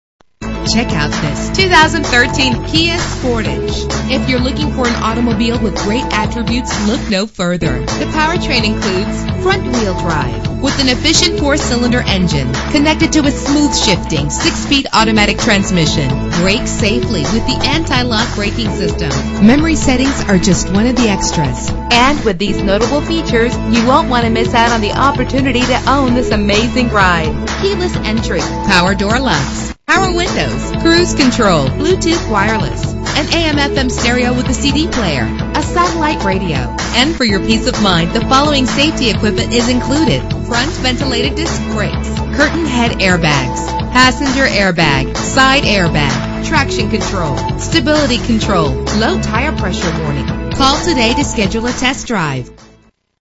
2013 Kia Sportage - Willowbrook IL
Year: 2013
Make: Kia
Model: Sportage
Trim: LX
Engine: 2.4 liter inline 4 cylinder 16 valve
Transmission: 6-Speed Automatic
Color: Bright Silver
Mileage: 5
Address:
7301 S. Kingery Hwy Rte 83